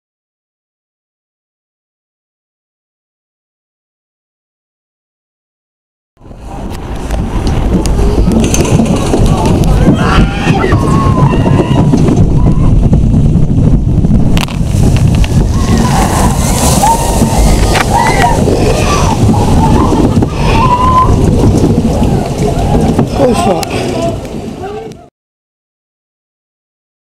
Tidal Wave - Thorpe Park - POV - 5|2023
A POV of Tidal Wave at Thorpe Park! Sorry the camera was a bit out of focus...! Enjoy!